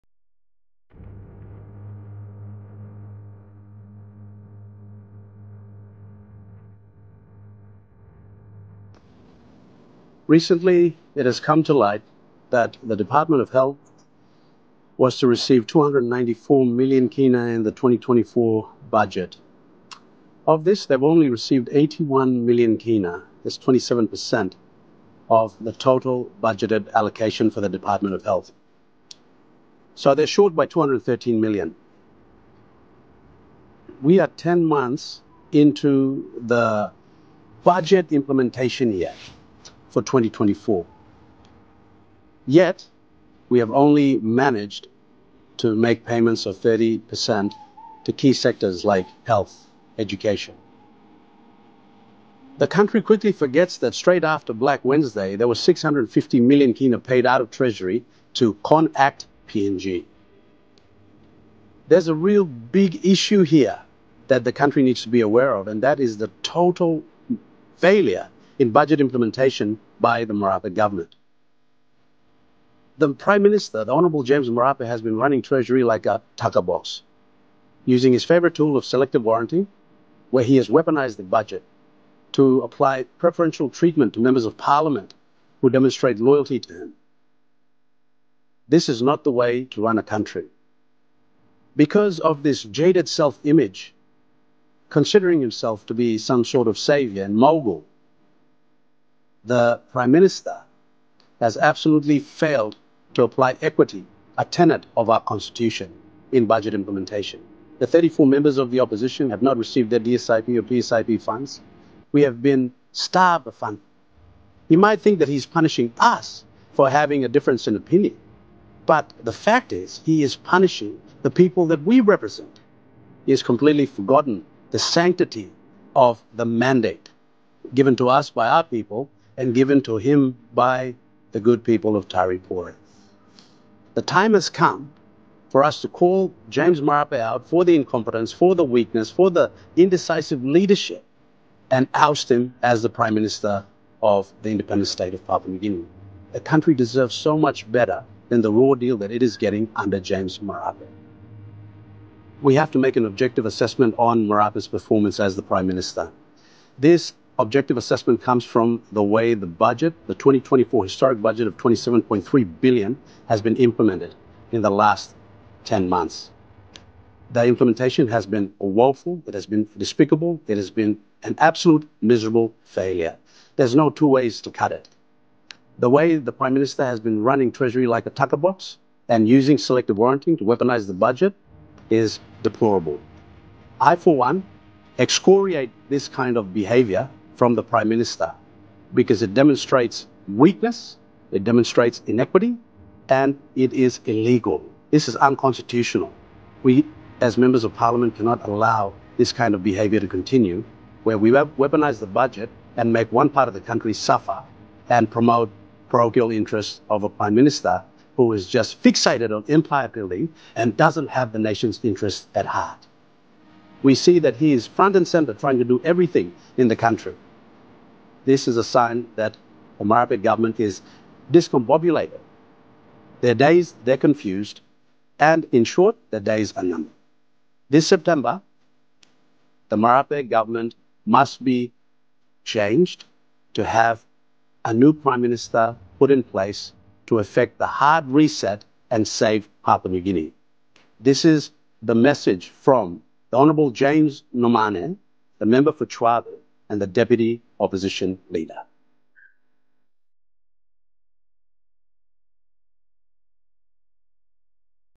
MARAPE FAILS TO IMPLEMENT 2024 NATIONAL BUDGET
For all the pomp and parade that went into announcing the historic budget K27.377 billion for 2024, the country is yet to see this budget translated into improved education and health services, safer communities, better sanitation and water supply, more jobs for the youth, and an increase in per capita income. The test for any budget’s impact is two-fold – in its financing and implementation.

The Marape government has three ways to access resources: taxation, borrowing, and printing money at the Central Bank. We have the highest taxes in history, we are overleveraged in our borrowing, and the collusion with the Central Bank in printing money through quantitative easing is worsening the cost-of-living crisis.

The Central Bank’s independence is compromised, and it cannot guarantee high employment, price stability, and low inflation. We must stop the insidious melding with the central bank and this elusive practice of printing money. The Marape government’s handling of its revenue sources has been counterproductive. We are more in debt, the ambitious tax regime is oppressive, and inflation has worsened the cost-of-living crisis. Financing of the 2024 Budget has been unsuccessful.

For budget implementation, let’s start with the chronic medical drug shortage affecting our health sector, reflected by the fact that from the K294 million budget allocation for Department of Health (DoH) in 2024, only K81 million (27%) has been paid to date, leaving K213 million outstanding. Our people continue to die from preventable diseases like malaria, typhoid and tuberculosis (TB) because of total funding shortage to DoH.

The crux of the implementation debacle is that we have 30% budget implementation after 10 months! We have only 2 months left before we announce the next budget. This budget implementation is deplorable and questions how we have prioritized and spent public monies? Marape has proven that he does not have the wherewithal to adhere to the Constitution, abide by the Public Finance Management Act, and effectively implement the 2024 National Budget. This is clear evidence of total incompetence.

On 22 August 2024, the International Monetary Fund (IMF) recklessly gave K470 million to the Marape government. Will the Prime Minister use this money to make payroll for public servants or pay the balance K213 million into the health sector? Or will he use this money for self-preservation with the imminent vote-of-no-confidence in September 2024?

The Prime Minister Hon James Marape is making PNG become a failed state through his weak leadership, skewed moral compass, and jaded self-concept akin to African dictators who use public resources at whim and neglect their people. Marape must not forget that public monies belong to the people of Papua New Guinea. Marape is fixated on empire building and self-preservation, not on principles like equity, altruism, and justice.

I excoriate the use of selective warranting and weaponizing of the budget and the trend of making decisions for political expediency. The Marape government has ruined the implementation of the 2024 National budget. This is on top of the litany of “glitches” that are synonymous with this low-expectation government. We are destined to become a banana republic if Members of Parliament allow this incompetence to continue.

In 10 months the Marape government has only achieved 30% implementation of the 2024 National Budget with total abuse and misuse of public monies outside of the 2024 Budget. Marape was unable to finance the K27.344 billion through taxation and borrowing. Secondly, the implementation has been riddled with corruption and is a total indictment on the Prime Minister, Hon James Marape. For example, look at the K17 million tablets that vaporized into thin air for the National Census. Why is this incompetent Minister still in Cabinet? Why have the police not arrested him and his Yoga instructor for misappropriation? These are tip-of-the-iceberg reasons for the Prime Minister to simply resign.

Prime Minister James Marape has already admitted that his government has no solutions and is unable to manage the affairs of the nation. This September, all Members of Parliament must decry this pathetic display of weakness and leadership focused on self-preservation. All MPs must not accept the Prime Minister’s acknowledgment of his shortcomings but take it one step further to depose him and vote for a new Prime Minister. We must rise to save Papua New Guinea. The 2024 Budget has failed. Under this government, all subsequent budgets, programs, and projects will also fail.

God bless all Members of Parliament to make the right decision in the national interest.

*ENDS*
Authorised for release by:

Hon. James Nomane, MP
Deputy Leader of the Opposition
Shadow Minister for Treasury, Finance, Economy and Rural Development
Member for Chuave